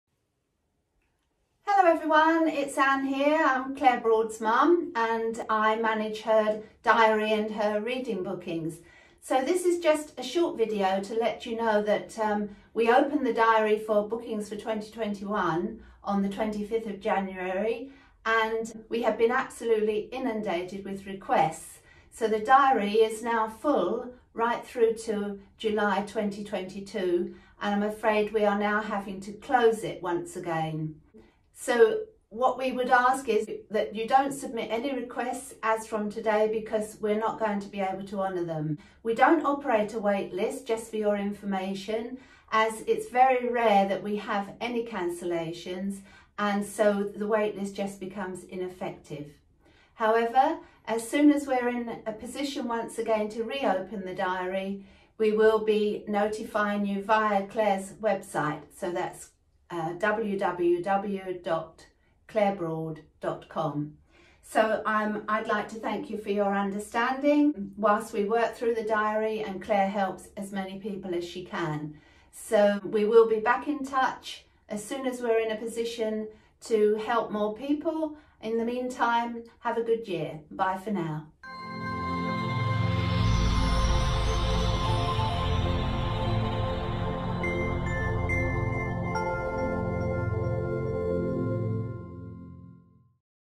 READINGS UPDATE 2021 - We have been inundated and have had to close the diary again already!
My goodness! The diary opened 25 Jan 2021 and Claire has been inundated with requests for readings.  We have received literally hundreds of requests in the space of a few short hours.  Unfortunately, in order to honour those requests, we now need to close the diary again until Claire works through all these commitments.

Thank you for your understanding x